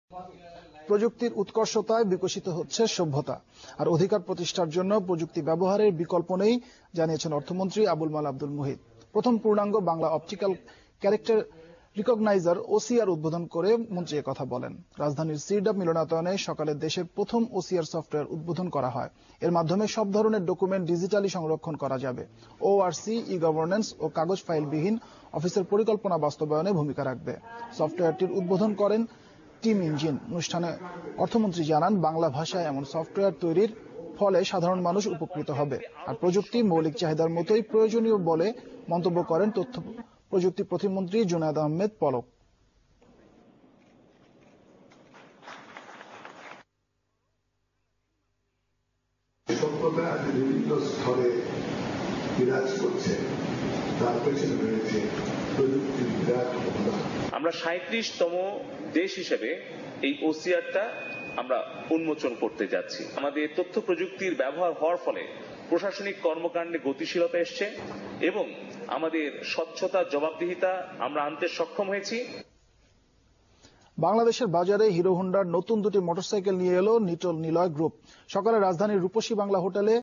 Puthi Bangla OCR(Optical character recognition) news on JTV
LAUNCHING A GLOBAL INNOVATION - First Complete Bangla Optical Character Recognizer (OCR) as technology and software
Puthi, world’s first-ever completed Bangla Optical Character Recognizer, was introduced publicly and launched on 7th August 2014 aiming to digitize, archive, search and edit all types of contents written in Bangla. Team Engine Limited, a Bangladeshi company, has launched the first complete Bangla OCR with a hope that this technology and software would support Bengali-speaking people and those interested in Bangla all over the world.
Bangla OCR, ‘Puthi’, can convert a page of a book within four seconds.  ‘Puthi’ currently can give 95 percent accurate information after the conversion from the main copy or text. ‘Puthi’ can now read more than 100 fonts now and the number would increase with continuous further development.
This Bangla OCR can digitize new-old, all types of books and documents written in Bangla.To find necessary informationit will not need hours to data entry or search manually anymore. Using the OCR, one just needs to search web or computer with the key words and will get the results. Bangla OCR is essential to build not only an online library, but also to implement e-governance in Bangladesh.
Words from Chief guest and Special guests – at the launching program
“Civilization is developed through thousands of small contribution from young people. Bangla OCR is such a contribution which has a far-reaching impact and will be changing Bangladesh. I congratulate Team Engine for this contribution.”- AbulMaal Abdul Muhith,Finance Minister, Government of Bangladesh.
“Puthi is a global innovation. From now, Bangladesh is 37th country in the world that is successful to make OCR. It’s a great pride for us. Even country like India could not develop Hindi OCR.”- Zunaid Ahmed Palak, State Minister, ICT Division, Government of Bangladesh.
“Bangla OCR ‘Puthi’ has opened a new horizon in the software industry of Bangladesh. It’s a milestone for the whole industry.”- Russell T. Ahmed, Senior Vice President, BASIS
“It’s very rare to see such type of home-grown innovation from Bangladesh. Innovating Bangla OCR is the manifestation of an extraordinary courage from Team Engine. -Mustafa Jabbar, Former President of Bangladesh Computer Samity.
“Innovating Bangla OCR is a great achievement for Bangladesh. Now Team Engine needs to ensure its copyright, IP for greater purpose and protecting their rights”-T I M NurulKabir, Secretary General of AMTOB
For Detail Information Please Contact-